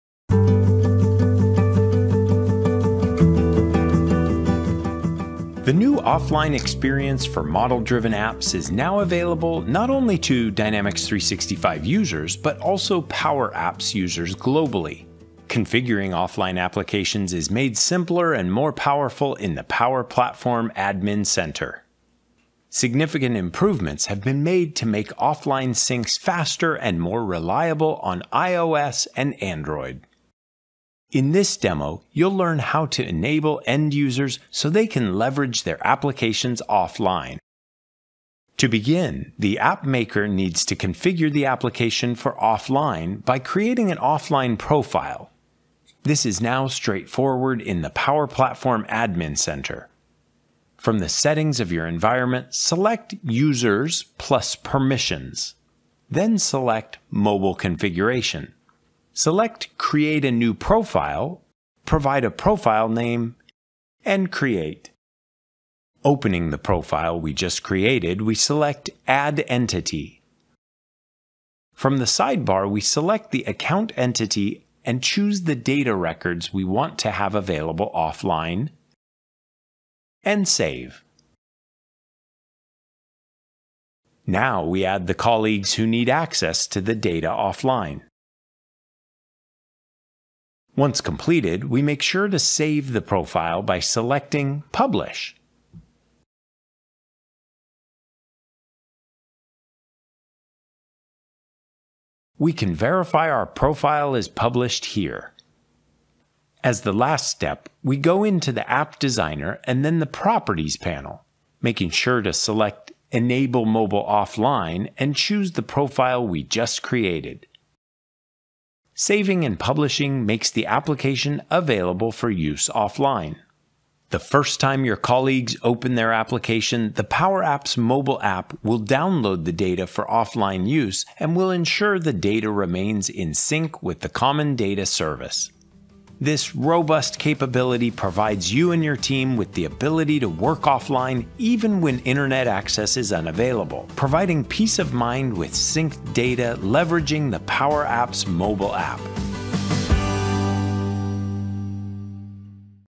Power Apps: Offline capabilities for model-driven apps
In this feature overview video series, we highlight new capabilities included in the latest update to Microsoft Power Apps.

The Power Apps mobile app provides an offline mode that lets you to work with your data even when you don't have internet access. Once you're back online, changes you've made are synchronized with your apps in Microsoft Dataverse.